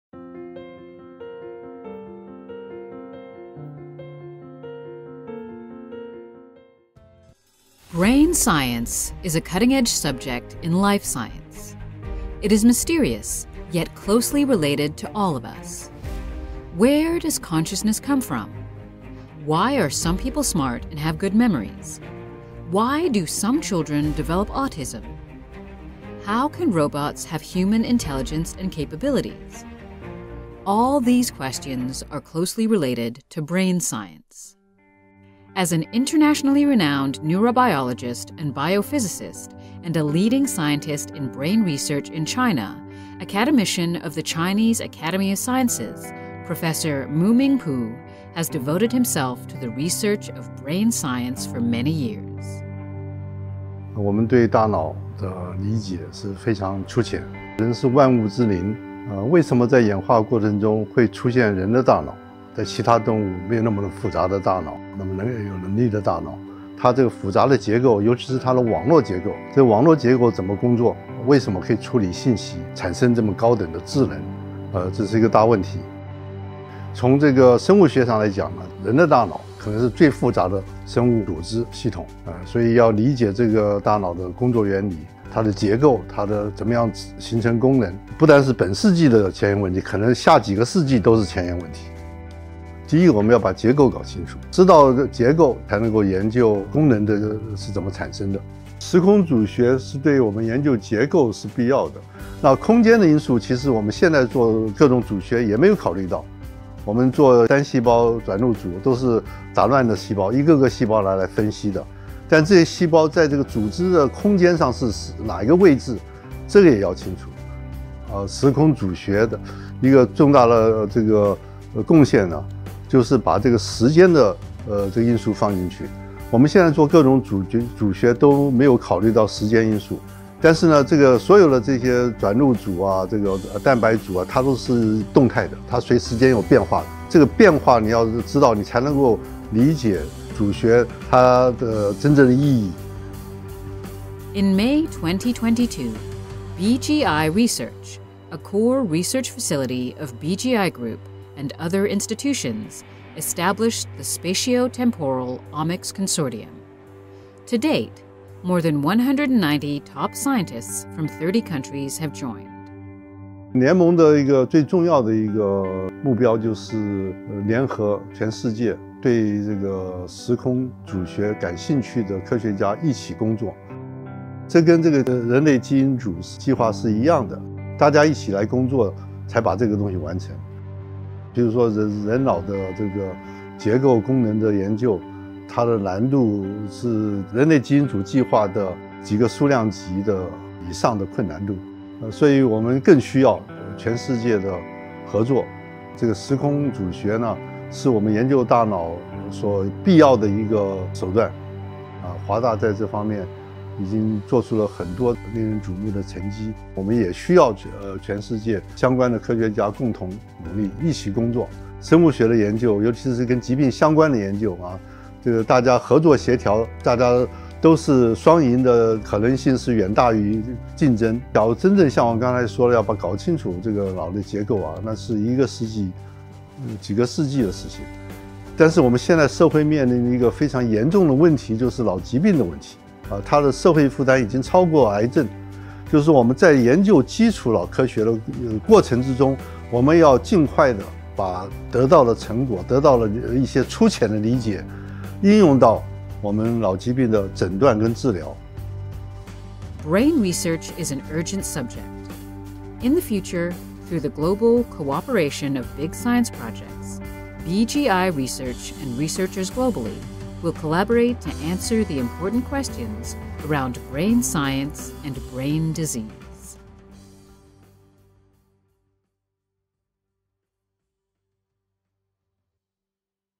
Professor Mu-Ming POO: Spatio-Temporal Omics is a Necessary Means for Us to Study the Brain
For thousands of years, human have been trying to understand a fundamental question about ourselves – What is inside our brain and how does it work? This many centuries-long topic continues to puzzle the world today, but thanks to global science collaboration and cutting-edge technologies, we are one step closer to demystify the most sophisticated organ in our body.

Among the greatest talents in the field of brain science, an internationally renowned neurobiologist and biophysicist, and a leading scientist in brain research in China, Academician of the Chinese Academy of Sciences, Professor Mu-ming POO has devoted himself to the research of brain science for many years.

In 2021, Professor Mu-ming POO led a team that cooperated with BGI Group and used BGI's spatio-temporal omics technology, Stereo-seq, to promote the research of the brain.

He also joined the SpatioTemporal Omics Consortium (STOC) initiated by BGI-Research and other institutions in 2022 as a core member to collaborate with more than 190 top scientists from 30 countries to answer some key questions in brain science.

In a recent interview, he shared his insights on brain science, spatio-temporal omics, and other areas.